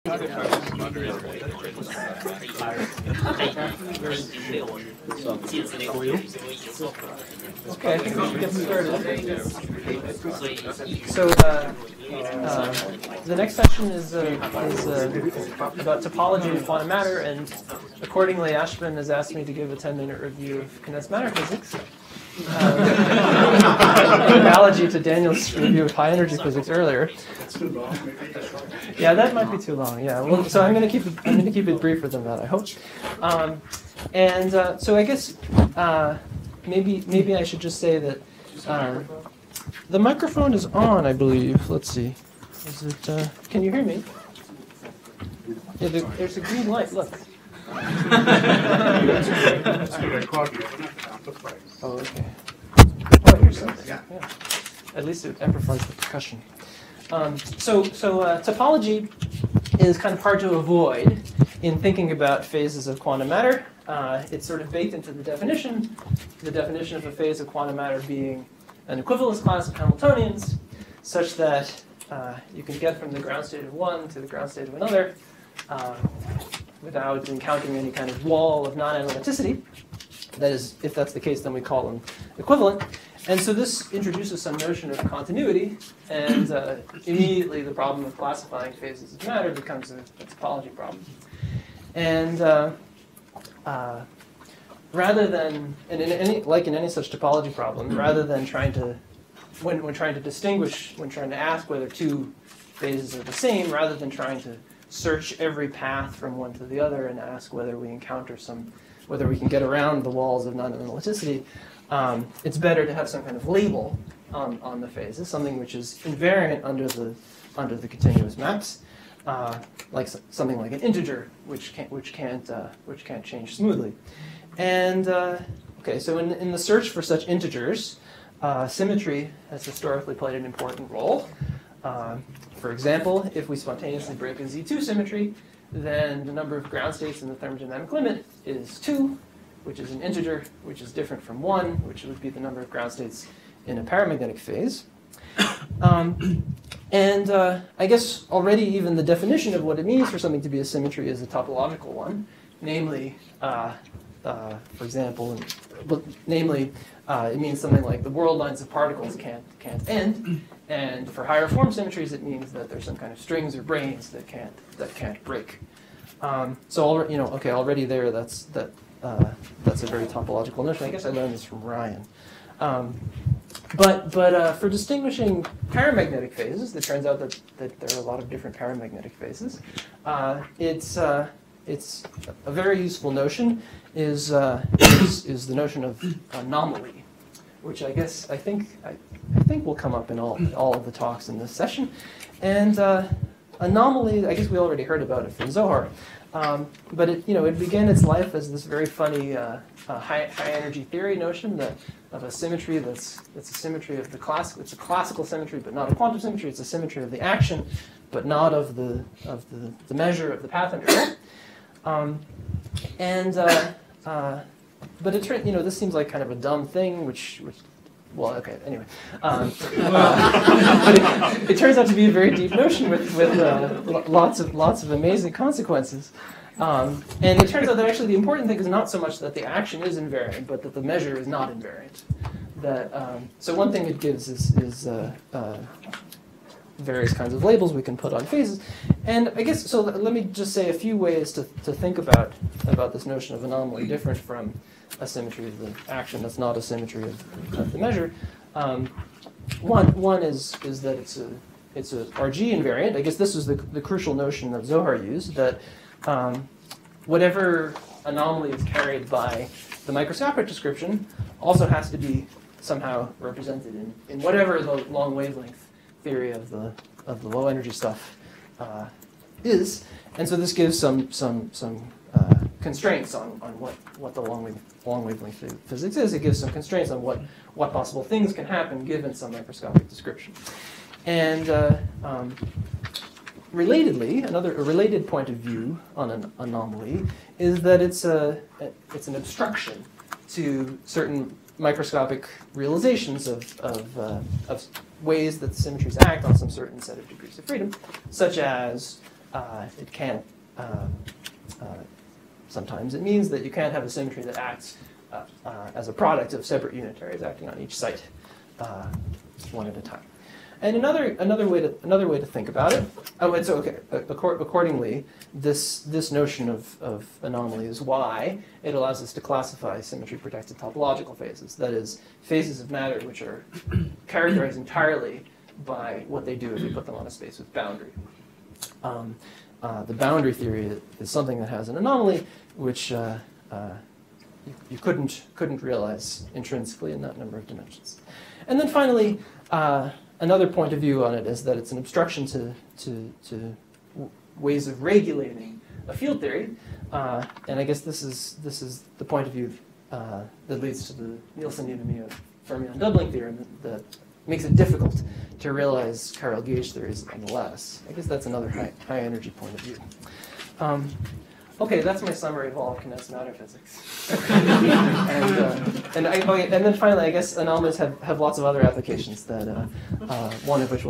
John McGreevy | Session 2 Introduction: Topological Phases in Quantum Matter, Pt. I
Speaker: John McGreevy, UCSD